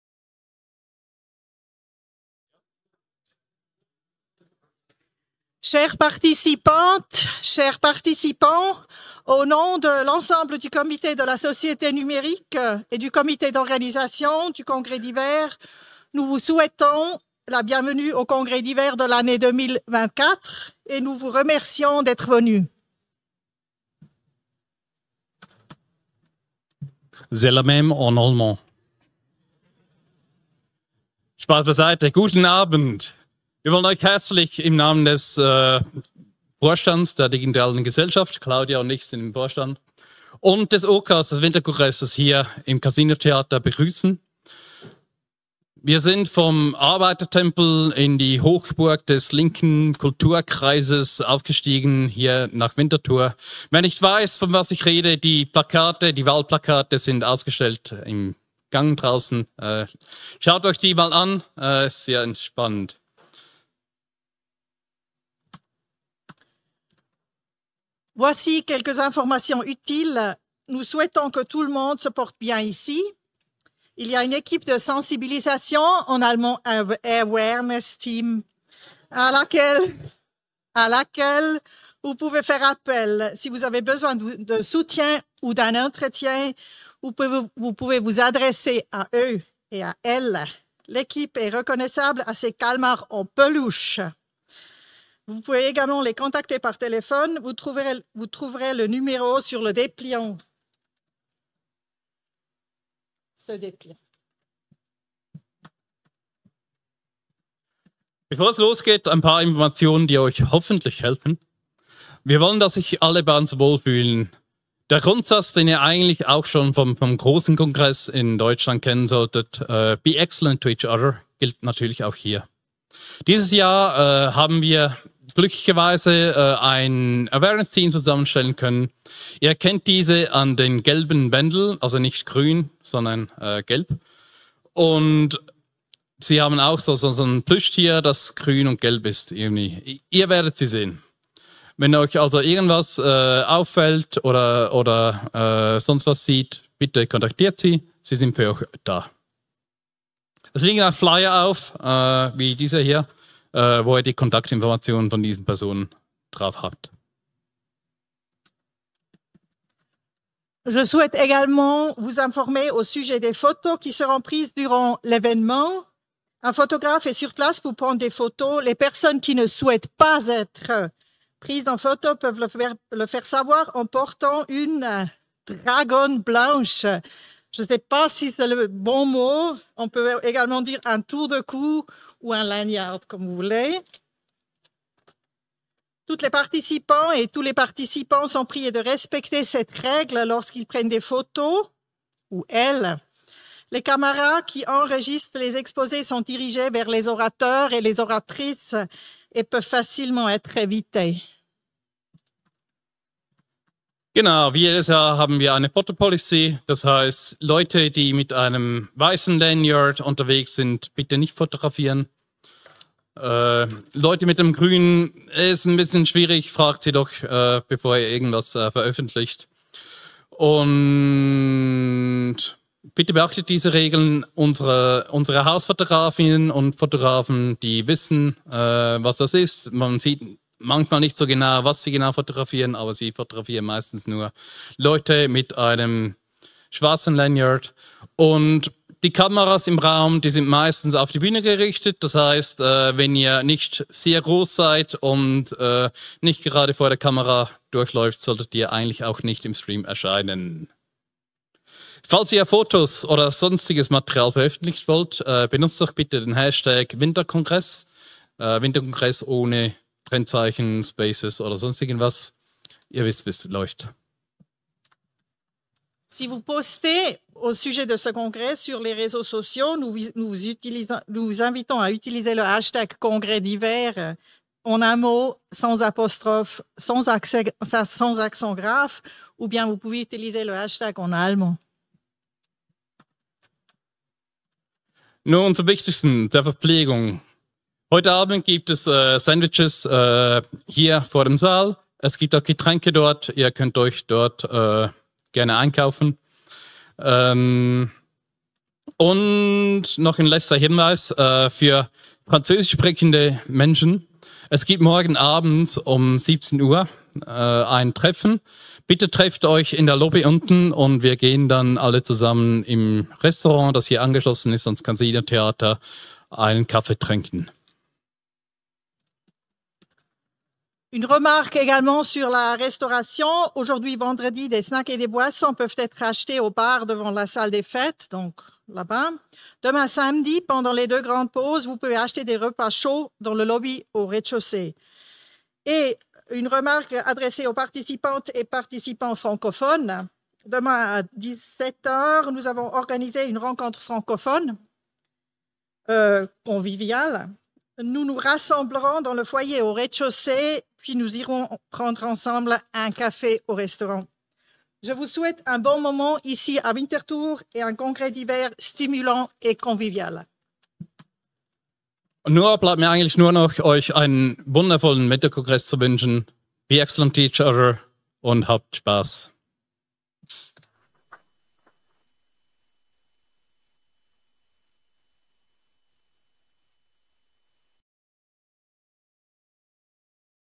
Eröffnung Winterkongress 2024 (Freitag)
Dies ist die Eröffnung am Freitag.

Winterkongress 2024
Recorded by chvoc & winkekatze.tv
Supported by Choas West TV & C3VOC